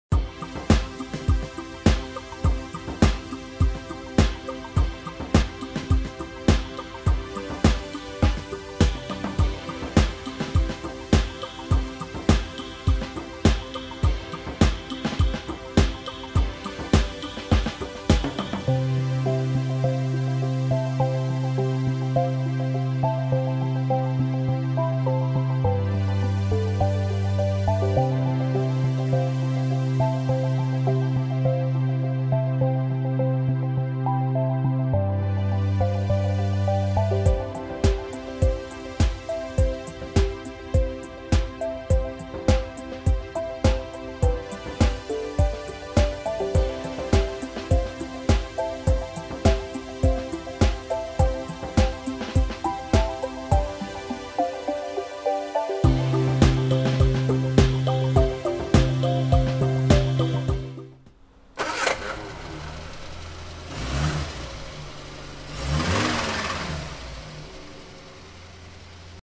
1957 Morris Minor Styleside Utility - 2022 Shannons Autumn Timed Online Auction
The 2022 Shannons Autumn Timed Online Auction running from 17 May to 24 May 2022 features a 1957 Morris Minor Styleside Utility.

- Ground-up restoration 2006-07
- Superb wooden load area
- Utterly charming and practical classic workhorse